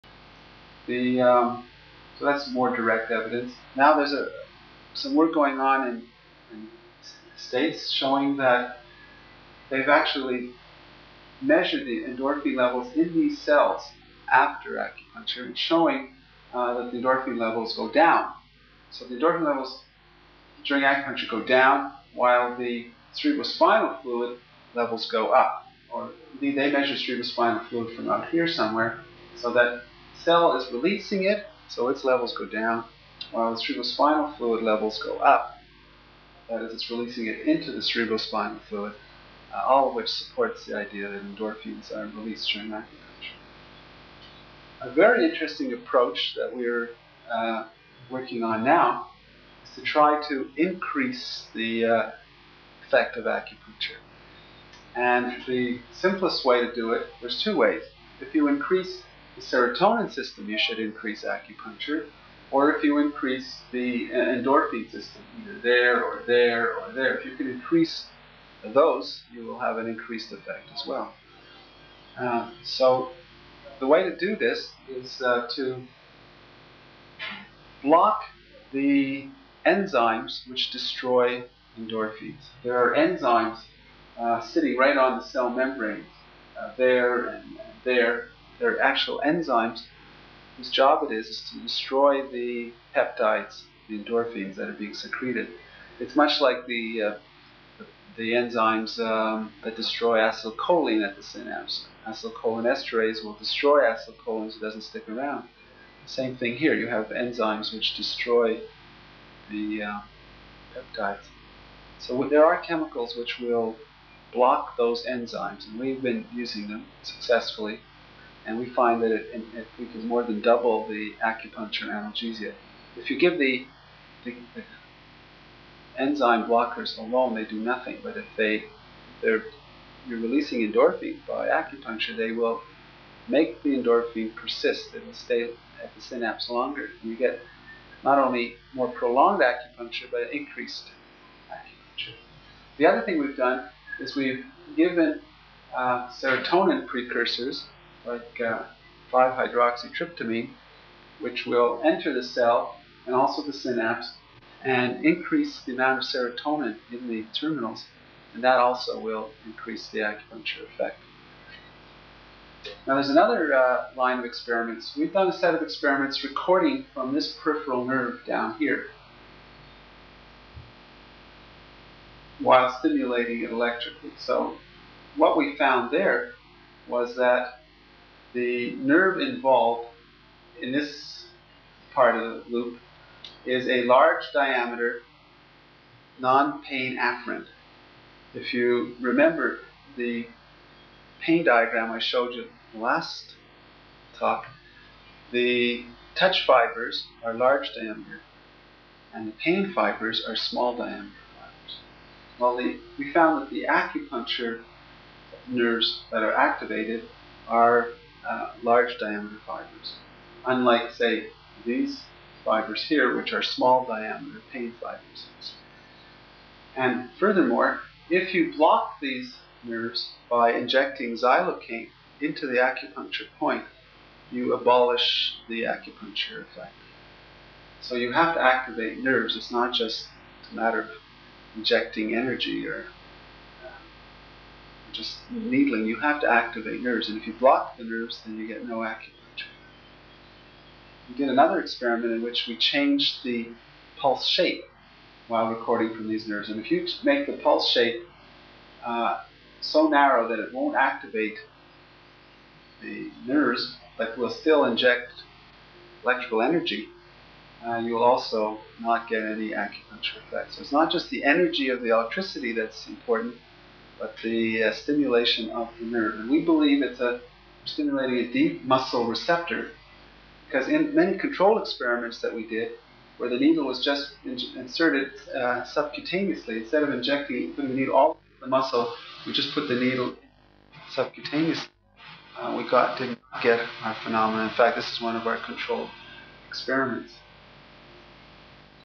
Acupuncture research Prof. Pomeranz lecturing 4/8
Prof. Bruce Pomeranz: Talk on neurophysiological basis of Acupuncture - Endophins involved -
Dept. of Physiology, University of Toronto 1987- Documented by Dr. Gabriel Stux